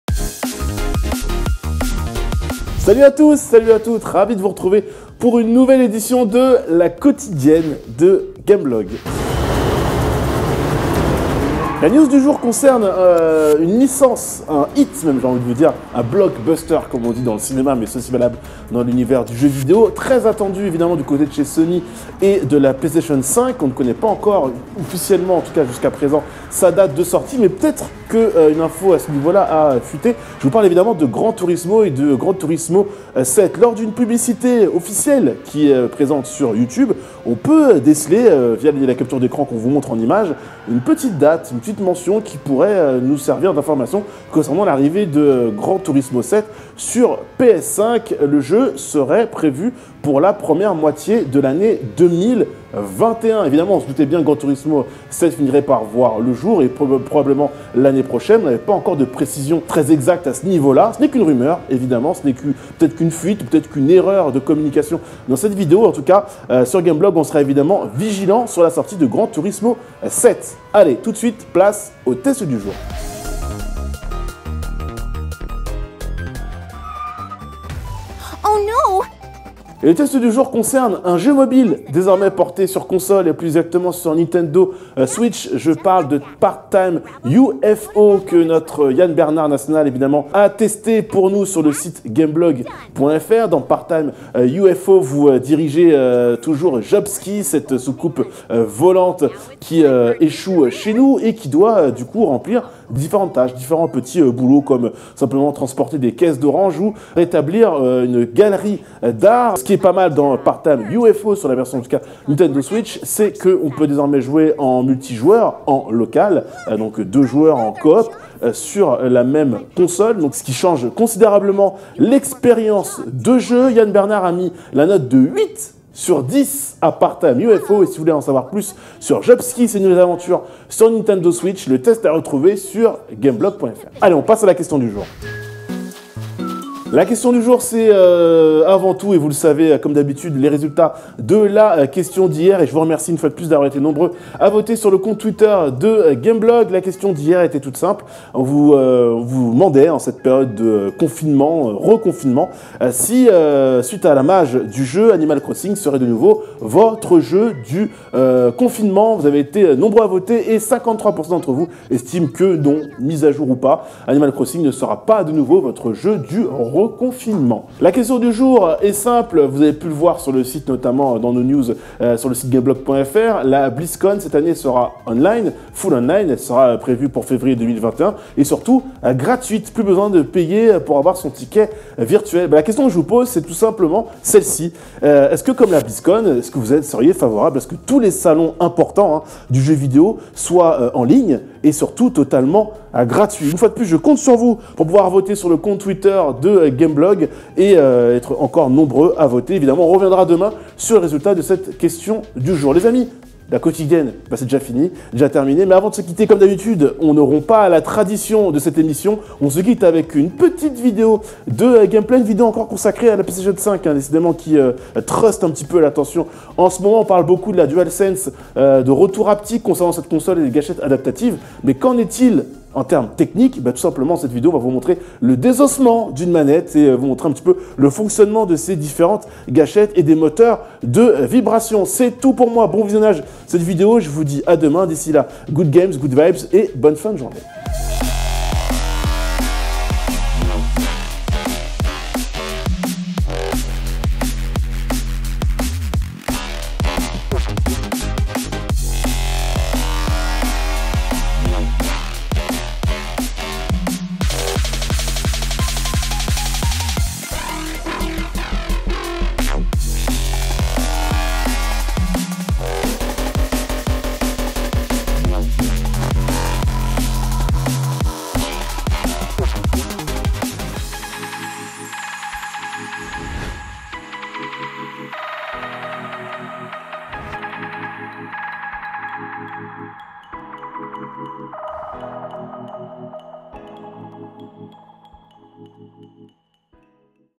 GT7 sur PS5, la Blizzcon en ligne et la DualSense décortiquée... La Quotidienne Jeu Vidéo #17 !
Retrouvez Alix et Thomas, nos deux animateurs attitrés qui vous accompagneront tout au long de la semaine !

SOMMAIRE :
00:00 • Générique + Introduction
00:10 • Gran Turismo 7 pourra déraper dès 2021 sur PS5
01:13 • Test du jour : Part Time UFO
02:15 • Question du jour : "À l'instar de la Blizzconline, êtes-vous favorable aux salons de jeu vidéo en ligne et gratuits ?"
03:22 • Vidéo du jour : La DualSense se fait décortiquer en tout bien tout honneur